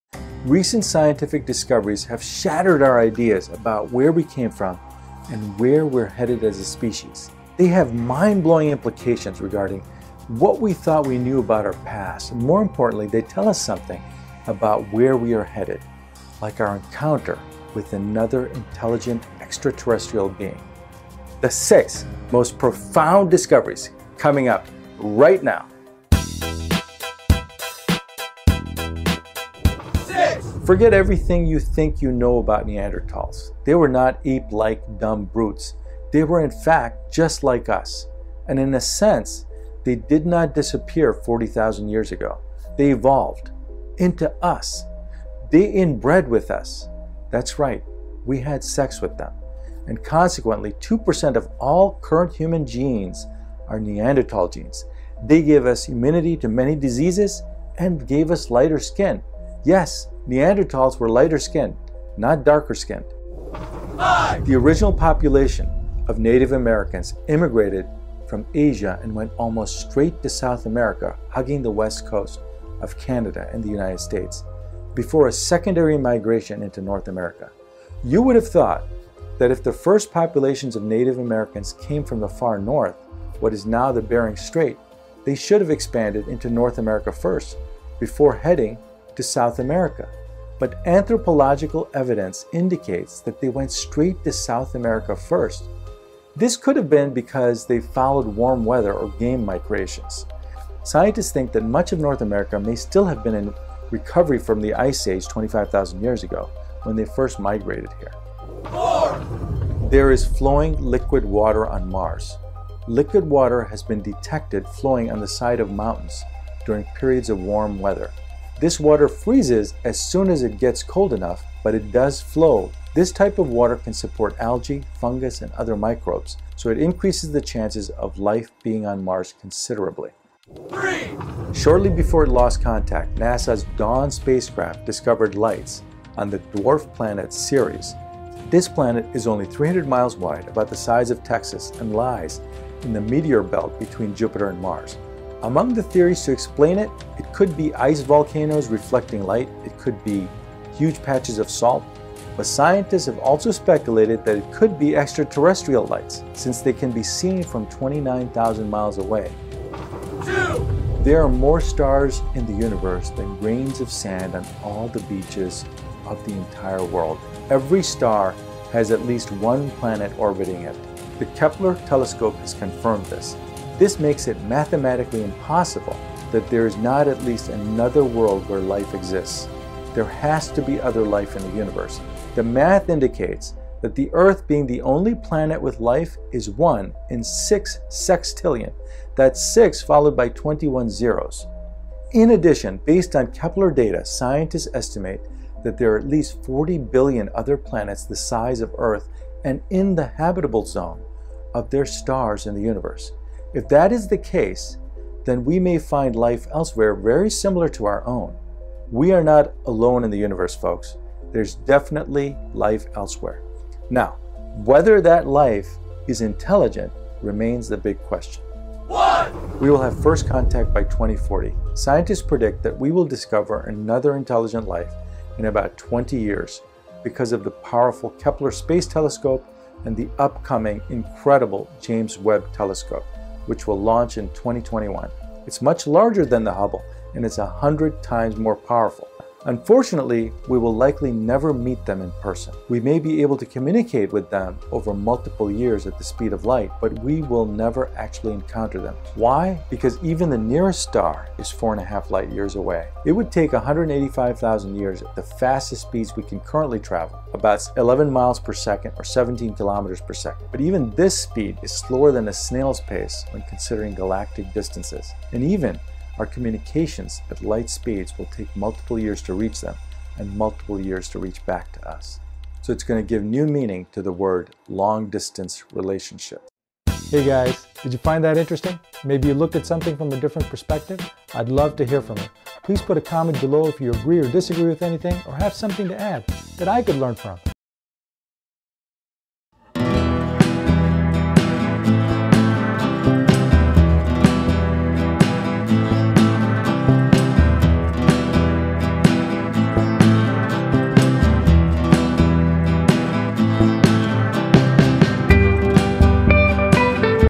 6 paradigm-shifting recent discoveries about humanity and extraterrestrial life
6 paradigm-shifting Discoveries About humanity and extraterrestrial life. Recent scientific discoveries have shattered our ideas about where we came from and where we are headed.  They have mind blowing implications regarding what we thought we knew about our past, and more importantly, they tell us something about where we are headed.

6) Forget everything you think you know about Neanderthals.  They were not ape-like, dumb brutes.  They were in fact, just like us.  And in a sense, they did not disappear 40,000 year ago.  They evolved – into us.  They interbred with us.  That’s right, we had sex with them… and consequently 2% of all current human genes are Neanderthal genes.  They gave us immunity from many diseases, and lighter skin colors.  Yes, they were lighter skinned than us, not darker.

5) The original population of native Americans immigrated from Asia and went almost straight to South America hugging the West Coast of Canada and the United States, before a secondary migration into North America.  You would have thought that if the first populations of Native Americans came from the far north, what is now the Bering strait, they should have expanded into North America first, before spreading to South America.  But anthropological finds indicate that they went straight to South America first.  This could have been because they followed warm weather or game migrations.  Scientist think much of North America may still have been in recovery from the ice age 25,000 years ago when they first migrated there.

4) There are more stars in the universe than grains of sand on all the beaches of the world.  And Every Star has at least one planet orbiting it. The Keppler telescope has confirmed this.  This makes it mathematically impossible that there is not at least another world where life exists.  There has to be other life in the universe.  The math indicates that the earth being the only planet with life is 1 in 6 sextillion.  6 followed by 21 zeros.  Based on Keppler data, Scientists estimate that there are at least 40 billion other planets the size of earth, and in the habitable zone of their stars in the Universe.  If that is the case, then we may find life elsewhere very similar to our own here.  We are not alone in the Universe folks.  There is definitely life elsewhere, but whether any life is intelligent remains the big unknown.

3) Shortly before it lost contact, NASA’s Dawn spacecraft discovered lights on the dwarf planet Ceres.  This planet is only 300 miles wide, about the size of Texas, and lies in the meteor belt between Jupiter and Mars. Among the theories to explain it: It could be ice volcanoes reflecting light.  It could be huge patches of salt.  But scientists have also speculated that it could be extraterrestrial lights, since they can be seen from 29,000 miles away by NASA’s Dawn.

2) There is flowing liquid water on Mars. Liquid water has been detected flowing on the side of mountains during periods of warm weather.  This water freezes as soon as it gets cold though.  This type of water can support algae, fungus and other microbes.  So, it increases the chance of life on Mars considerably.

1) We will have first contact by 2040. Scientists predict that we will discover another intelligent life in about 20 years because of the powerful Keppler space telescope and the upcoming incredible James Webb telescope which will launch in 2021.  It is much larger than Hubble, and is 100 times more powerful.  Unfortunately, we will likely never meet them in person.  We may be able to communicate with them over multiple years at the speed of light, but we will never actually encounter them?  Why? because even the nearest star to is 4.5 light years away.  It would take 185,000 years at the fastest speeds we can currently travel, about 17 kilometers/second, or 11 miles per second.  But even this speed is slower than snail’s pace when considering galactic distances. And even our communications at light speed will take multiple years to reach them, and multiple years to reach back to us.